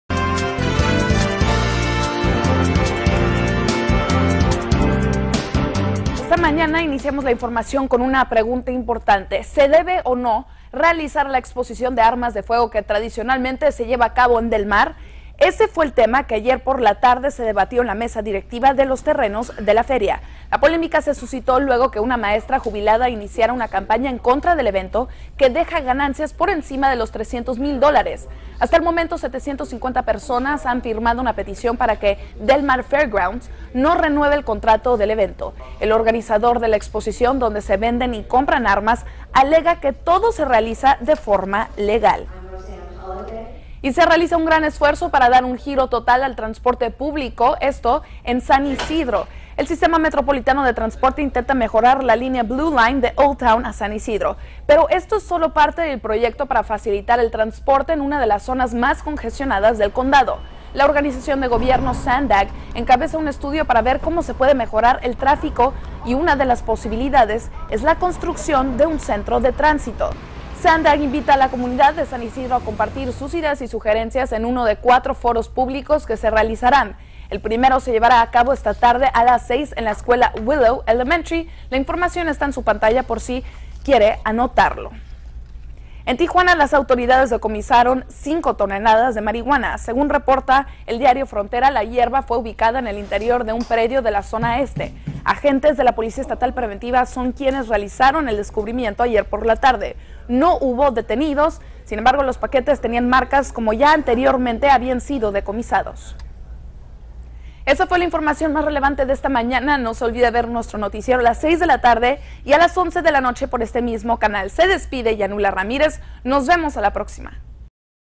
Giro completo a transporte público en San Ysidro
Foros comunitarios para mejorar trafico en la zona.